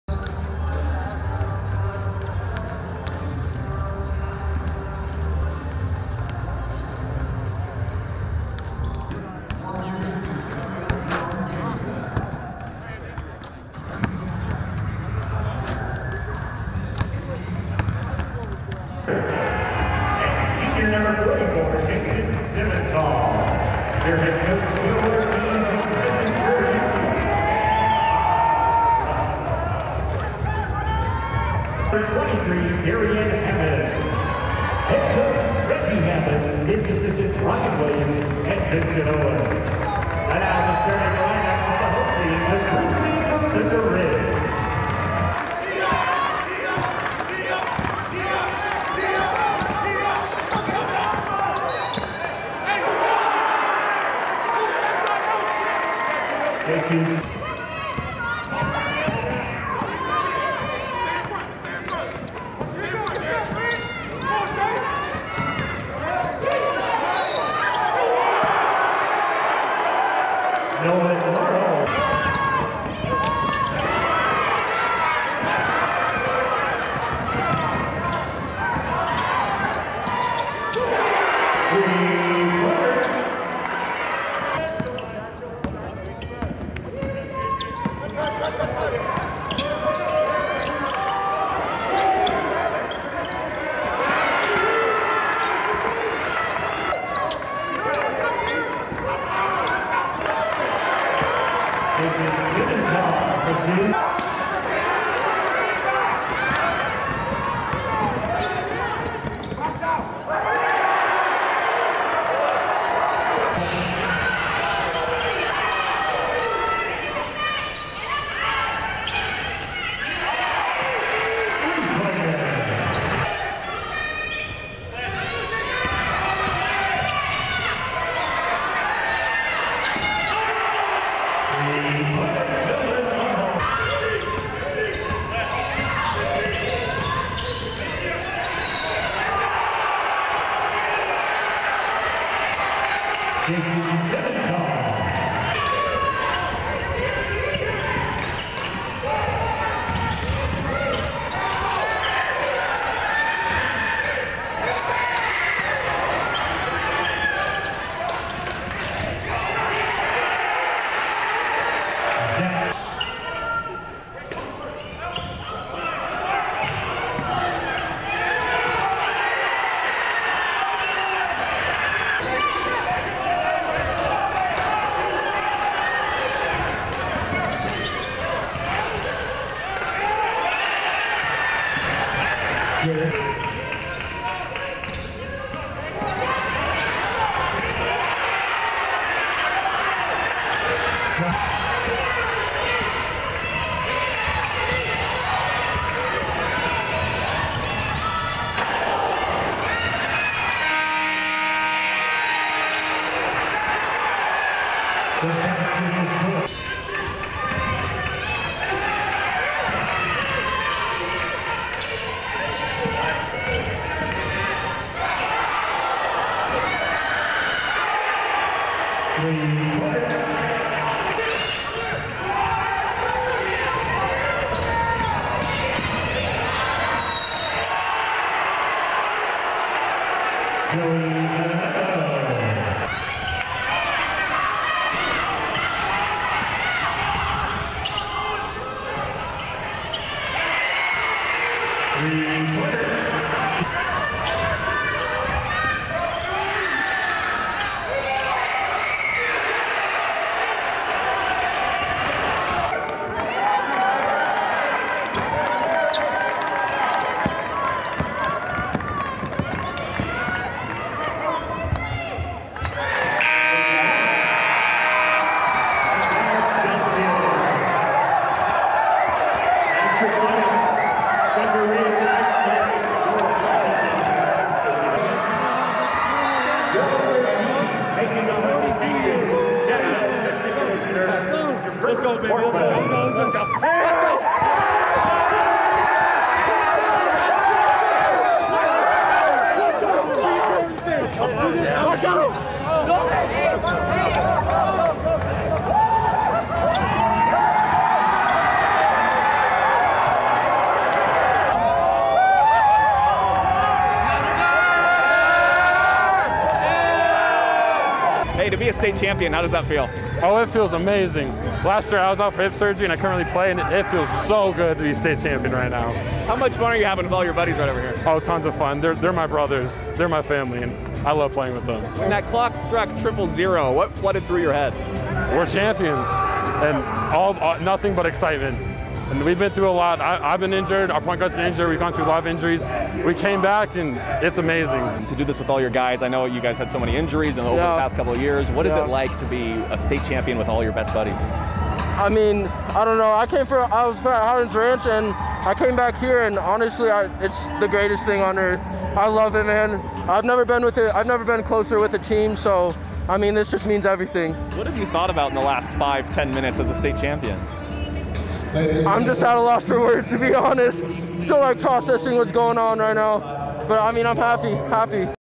ThunderRidge boys basketball claims 5A championship over George Washington
The Grizzlies took control in the second quarter and never looked back on their way to a 68-59 win.